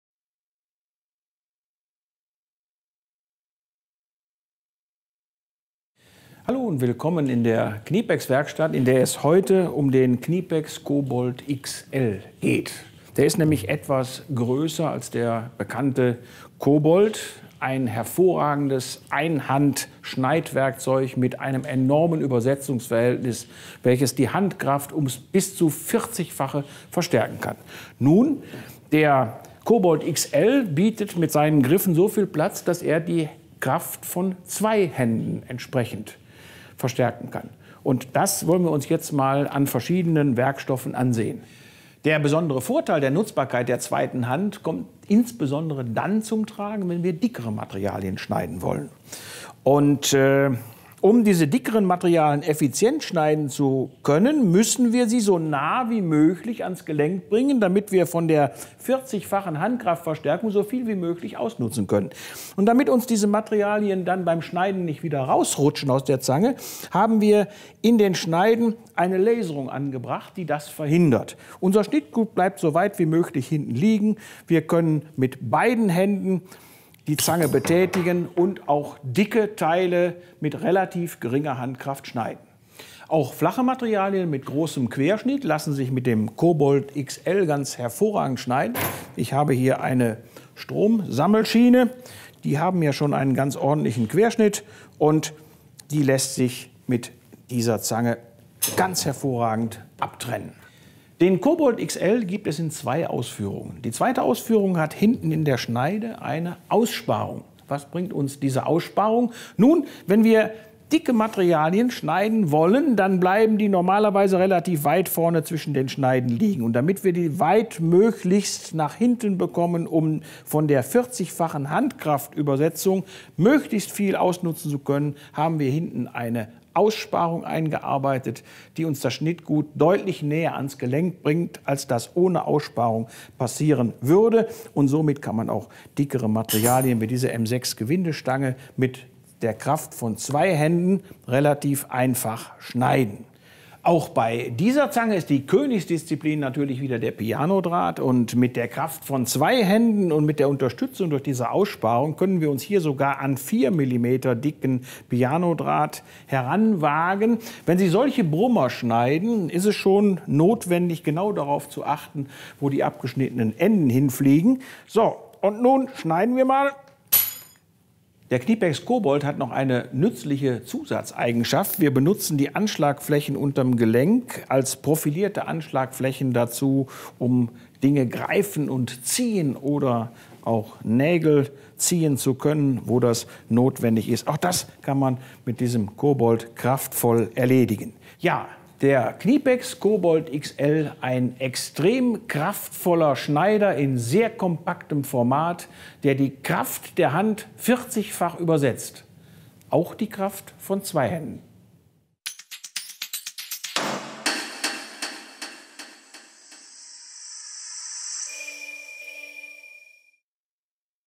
KNIPEX CoBolt® Kompakt-Bolzenschneider
Für kraftvolles Schneiden auch größerer Querschnitte und sehr harter Materialien. Zweihandbedienung für maximale Schnittkraft.
Geeignet für Materialstärken von 3,6 - 6,0 mm.
Art.Nr.: 720586